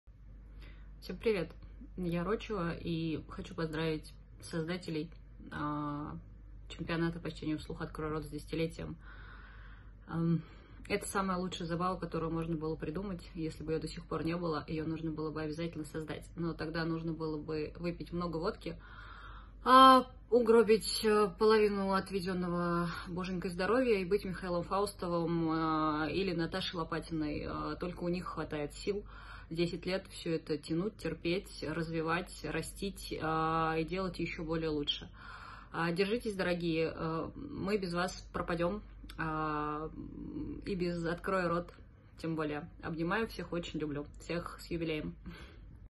Ирина Рочева поздравляет "Открой Рот" с юбилеем
Поздравление от Ирины Рочевой (книжный магазин "Свидетель" и фестиваль "ЛитераТула").

Ира, спасибо!